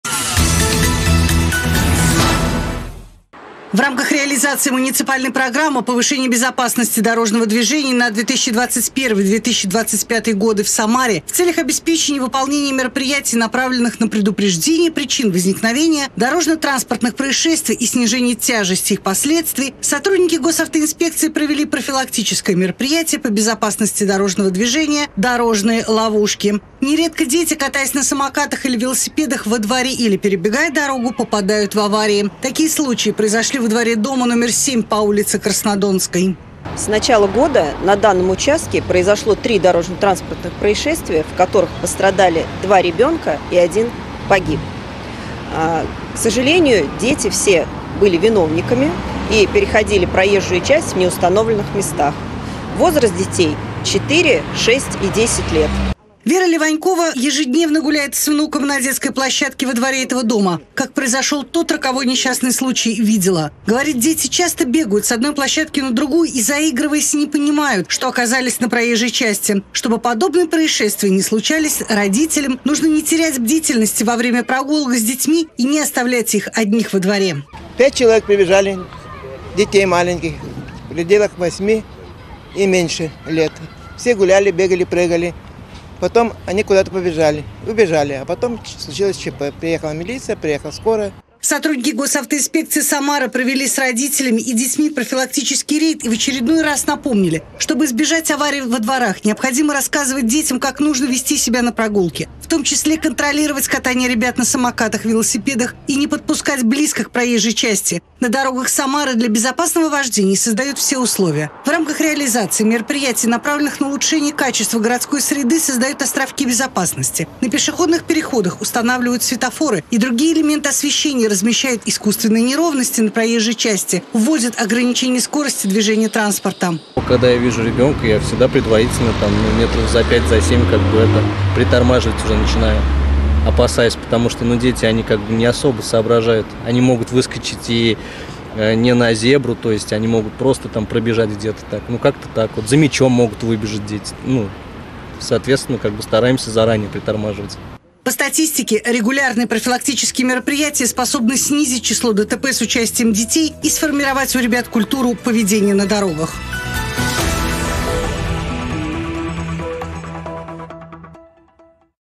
Специальный репортаж от 19.10.2023. "Профилактическое мероприятие "Дорожные ловушки"
В рамках реализации муниципальной программы "Повышение безопасности дорожного движения" на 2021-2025 годы в Самаре в целях обеспечения выполнения мероприятий, направленных на предупреждение причин возникновения дорожно-транспортных происшествий и снижение тяжести их последствий сотрудники Госавтоинспекции провели профилактическое мероприятие по безопасности дорожного движения "Дорожные ловушки".